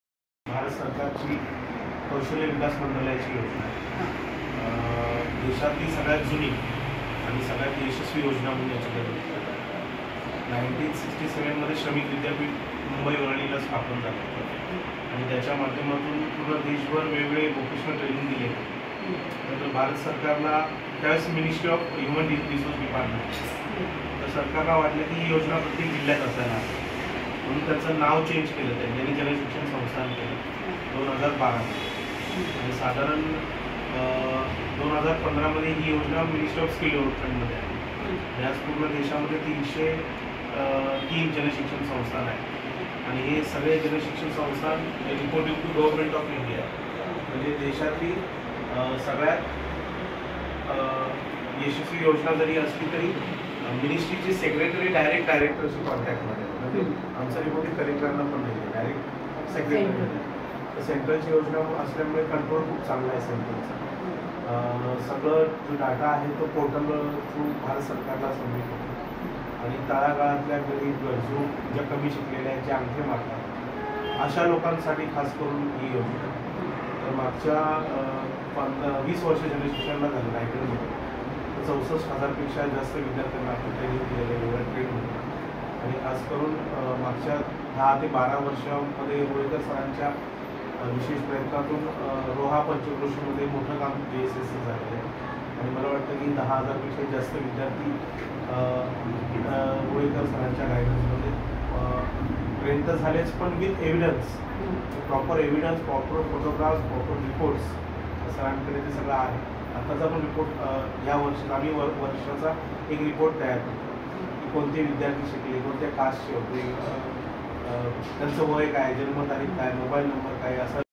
1.Jan Shikshan Sansthan Raigad Visited At. VRTI Roha Batch Visit & Course Promotion
Jan Shikshan Sansthan Raigad Visited At. VRTI Roha Batch Visit & Course Promotion For Cutting & Tailoring & Advance Beauty Parlour In Presence Of Dr. Vijay Kokane (Director), Mrs. Vedanti Patil, Miss. Rajashri Naik (Assistant Programe Officer), Mrs. Seema Gudale, Mrs. Meena Sandge (Resource Person) & All Representative Of Jan Shikshan Sansthan Raigad On Date:- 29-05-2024 (Excel Industries Ltd-roha CSR Through "Shree Vivekananda Research & Training Institute VRTI-ROHA")